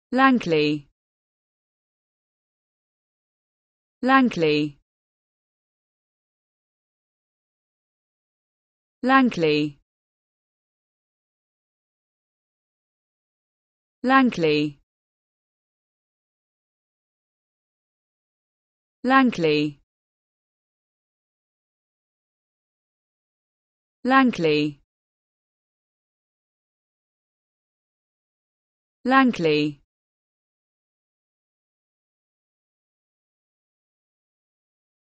the correct pronunciation of lantern in English.
This video shows you how to pronunciation lantern in English.